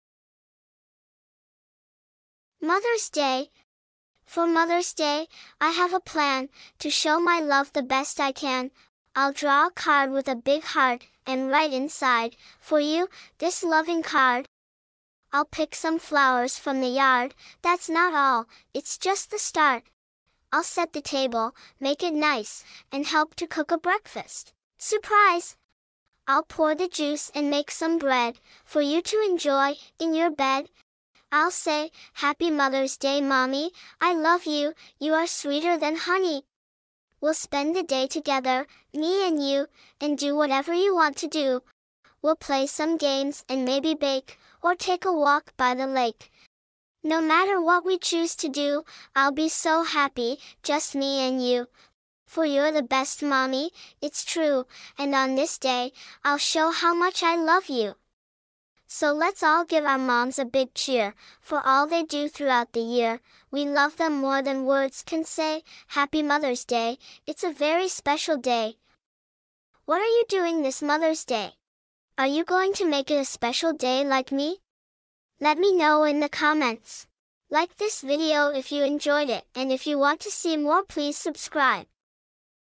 Happy Mother's Day poem
Hi, I'm The Poet Girl,

I like to make poems.
Today's poem is about Mother's Day. I love my mommy and want to do something special for her.